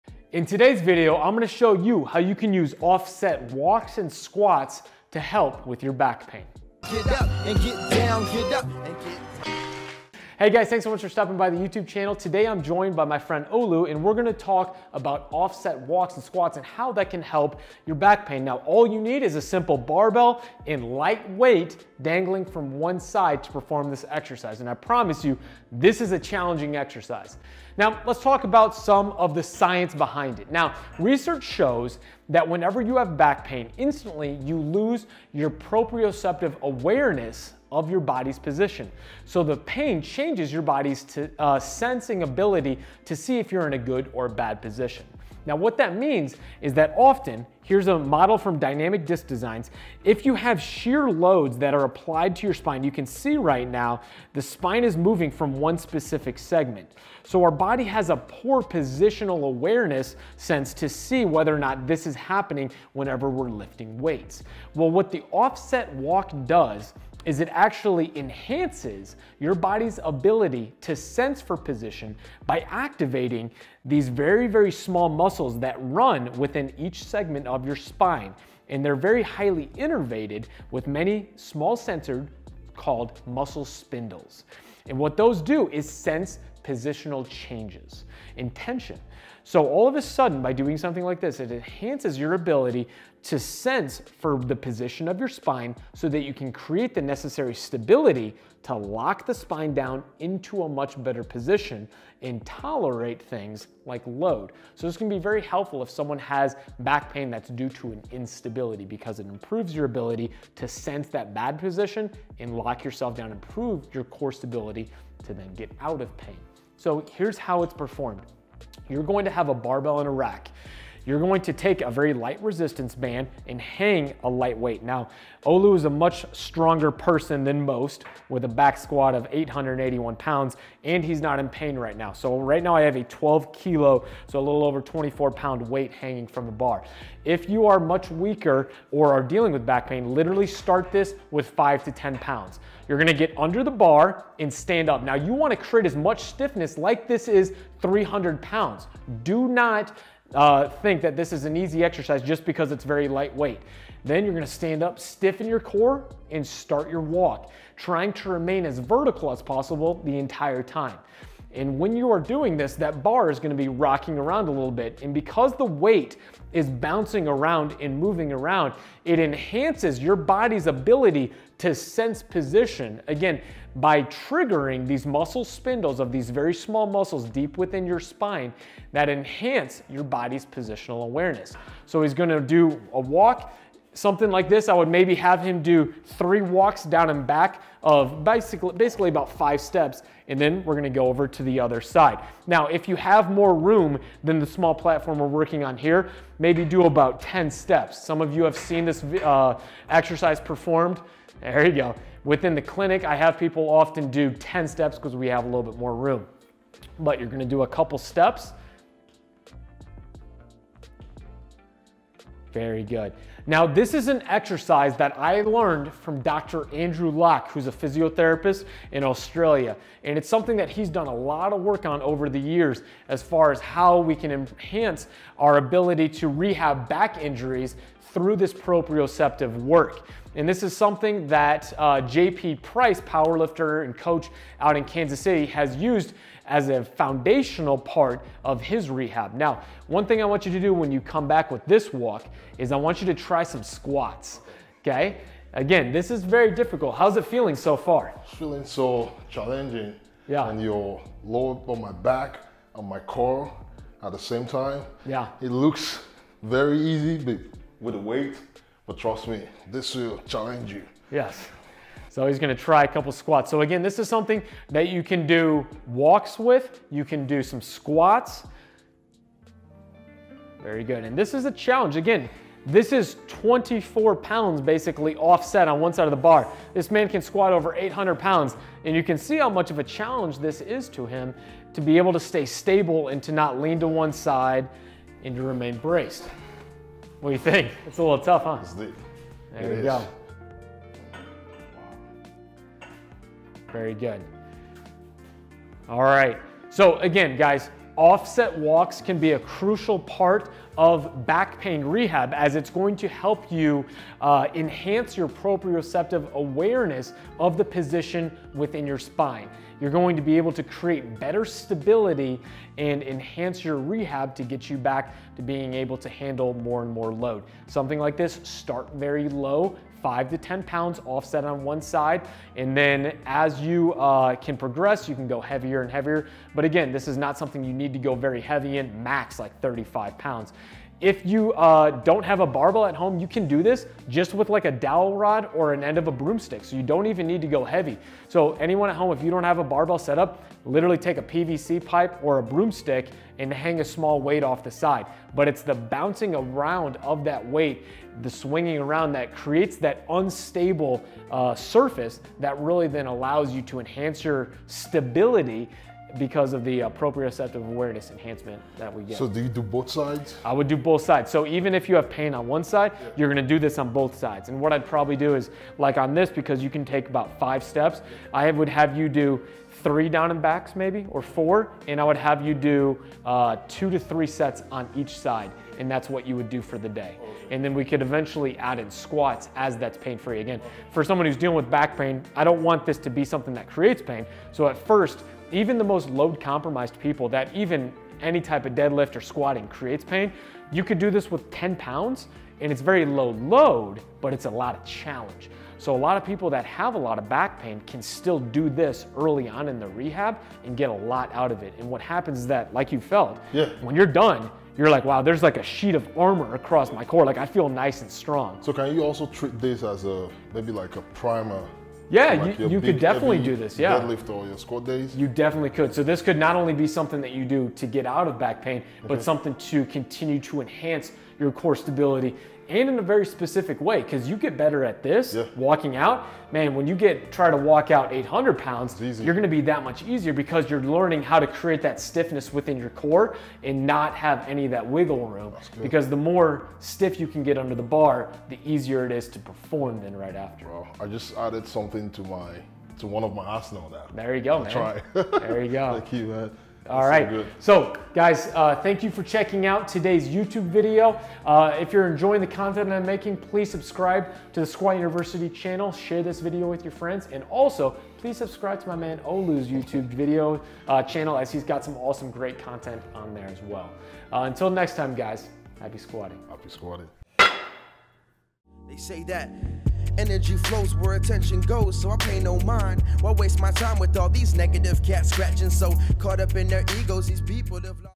Offset Walks & Squats for Back Pain (ENHANCE CORE STABILITY)
Fixing back pain is no easy task. While there is no one-size-fit-all plan that works for everyone, exercises that work to enhance stability and proprioception can often be very helpful for many. These exercises demonstrated by @olustrong-nigeriasstronges372 today have been popularized recently by Dr. Andrew Lock (be sure to follow him on Instagram today).

If you're currently dealing with low back pain, be sure to check out this blog to understand your cause for injury

The band I'm using to hang the weight is the green monster band from rogue

Shout out to Dynamic Disc Designs for the amazing model used in todays video